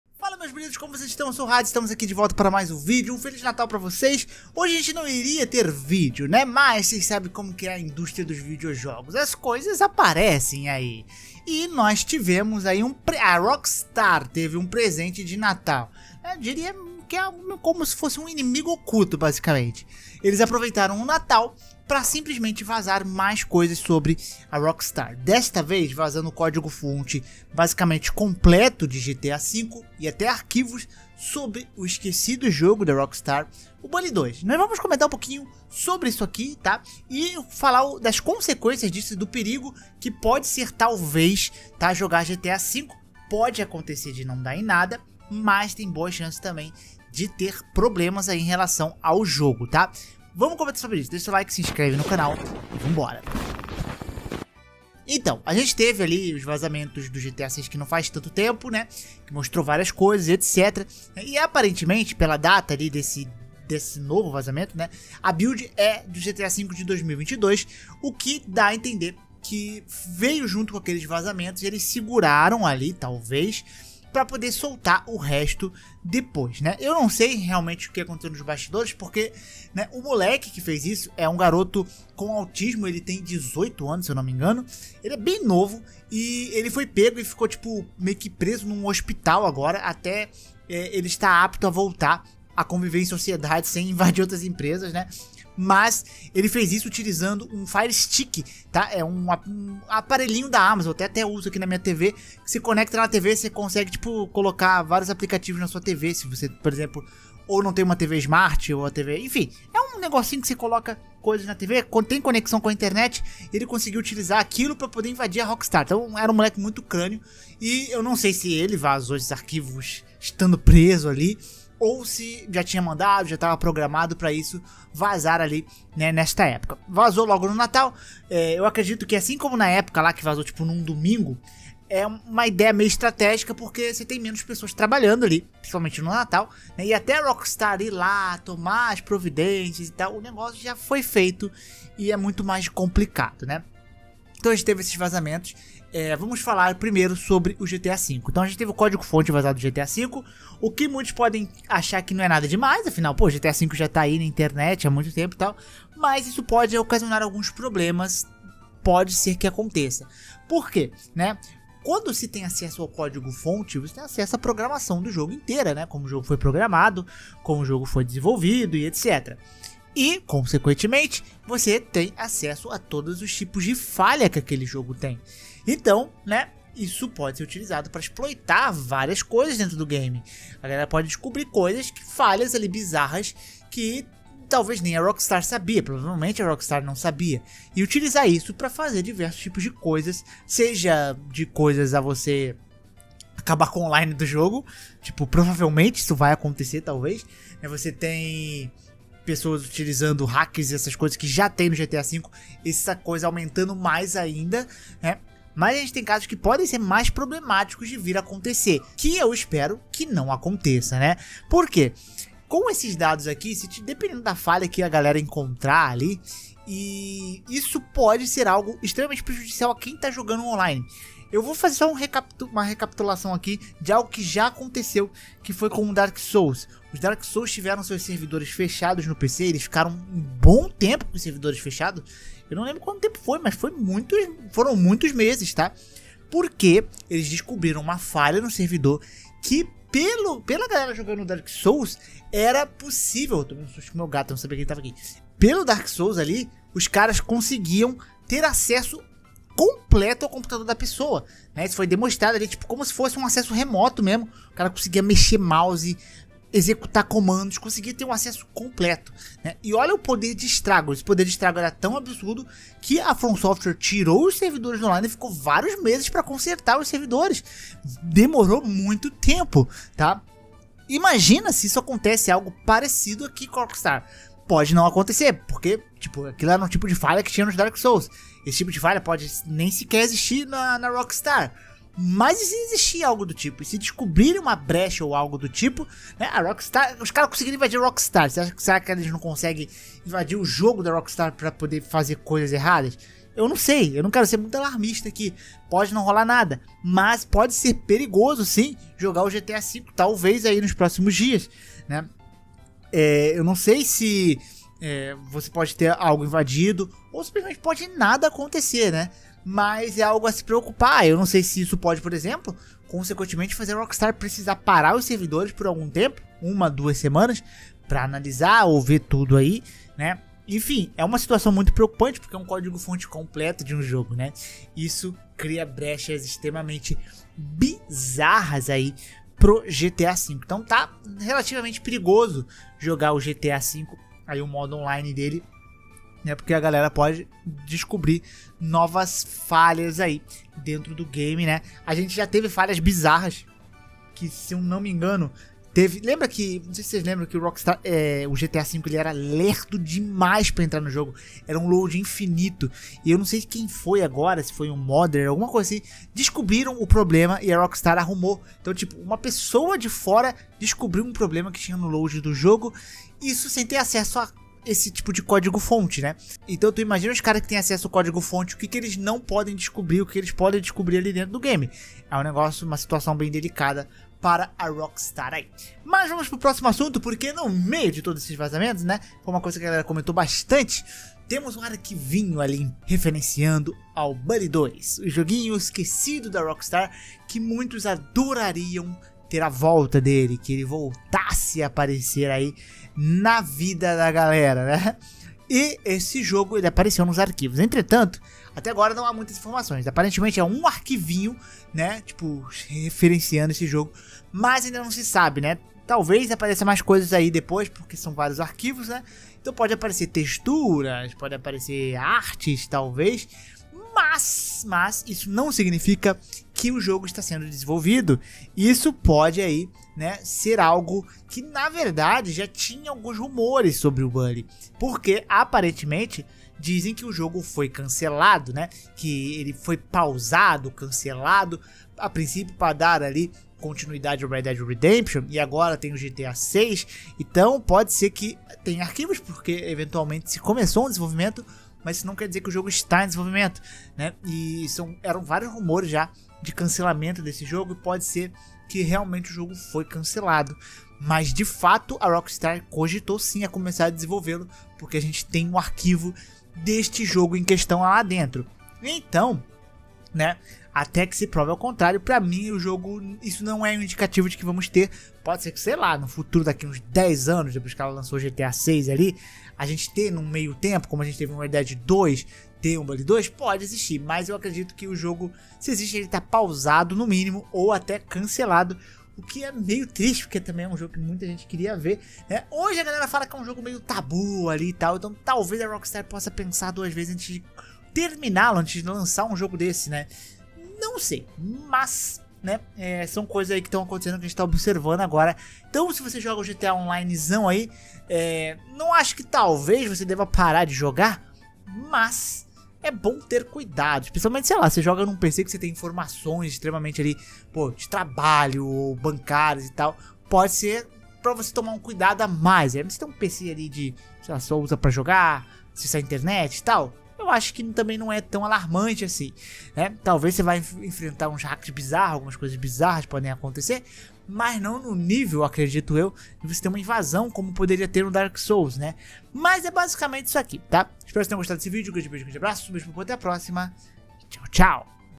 NOVO VAZAMENTO DE GTA e BULLY 2! GTA 5 é PERIGOSO agora?
Saiu coisas de GTA 5 e bull 2

➤ PSN: hadessama94

00:00 GTA 5
06:16 Bully 2